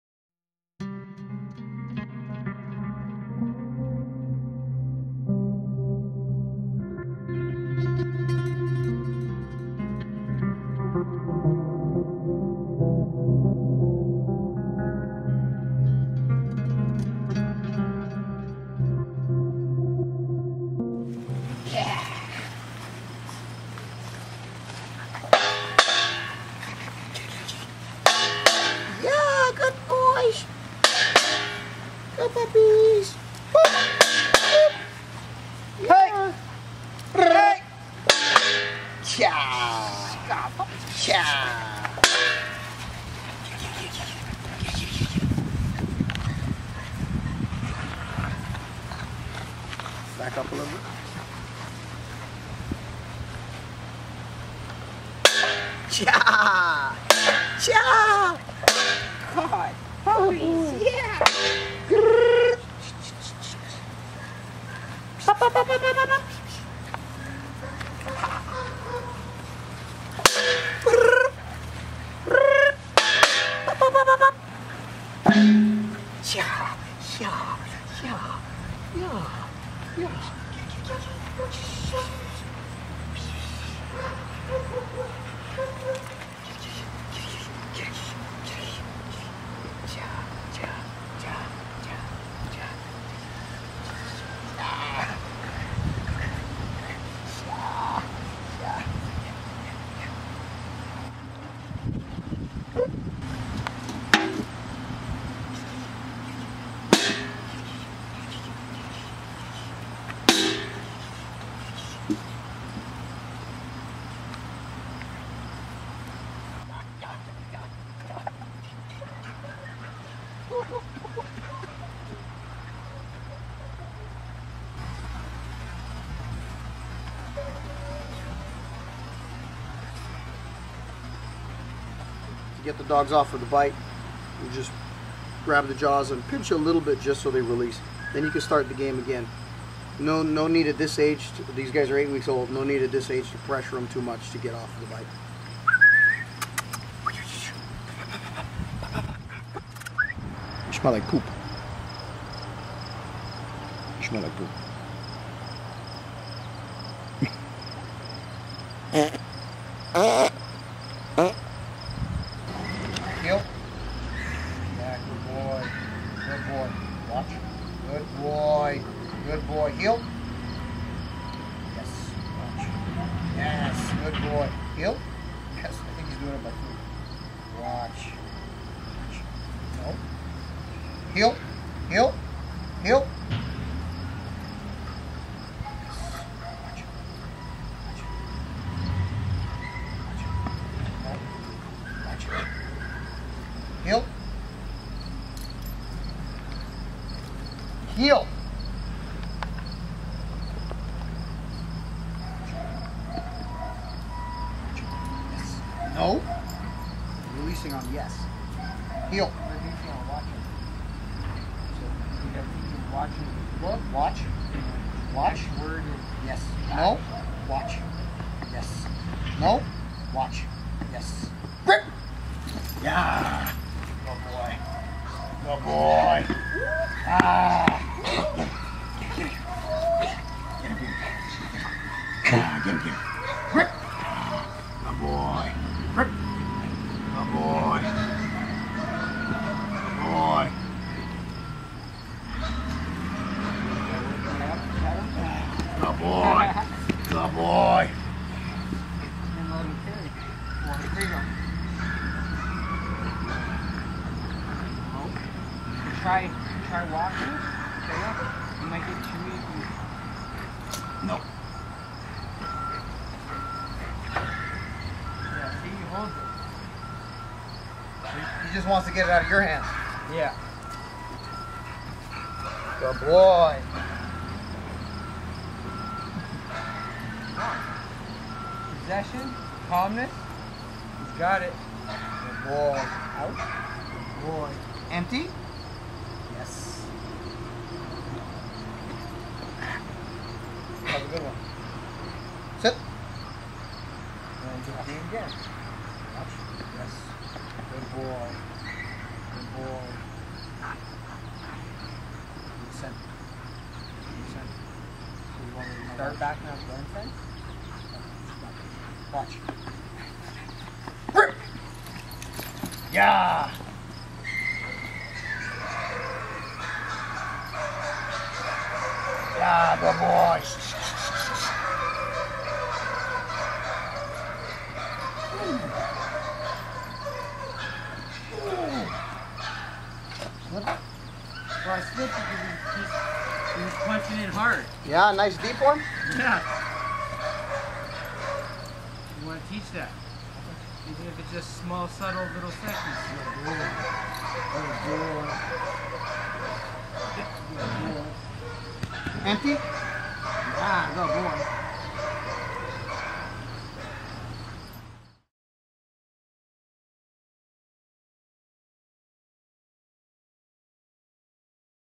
puppy building drive for pit bull bite work rag dog training sport protection
This is part 3 of building a base for your puppies bite work.  I am working Xena and Achilles monster pups and Atlas in the later development of his puppy stage.  Dogs don't develop over night and comes with confidence, drive, repetition, love and time.